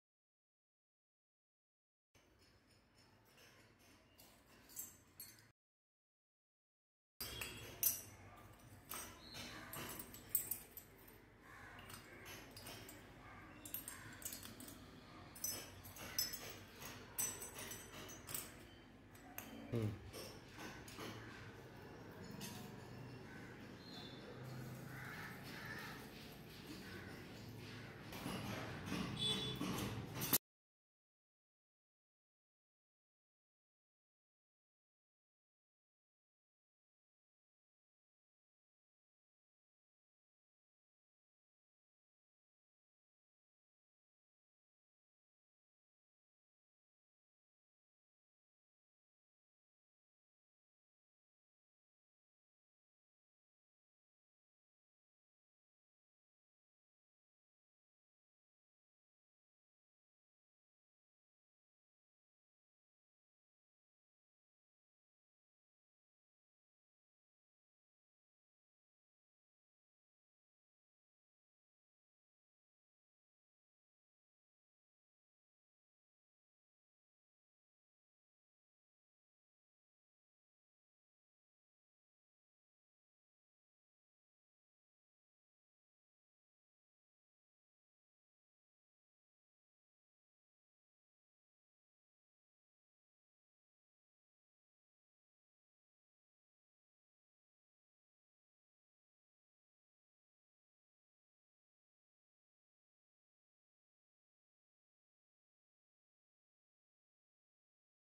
Amazing skill by a professional | purification of Copper metal | MELTING
Amazing skill by a professional | purification of Copper metal | MELTING | How to melt copper and how to purify it using ancient method!!

Really amazing.. skilled professional caster melting copper in high flame and purifying them easily.
Copper Design Art - copper droplets done by professional metal caster. Don't try at home. Comment your view on the output design!

Attention: The person in this video is a professional MORE THAN 40 YEARS EXPERIENCED metal caster. Please do not try at home as it can be dangerous if you do not have any safety types of equipment and have little or no experience in this field.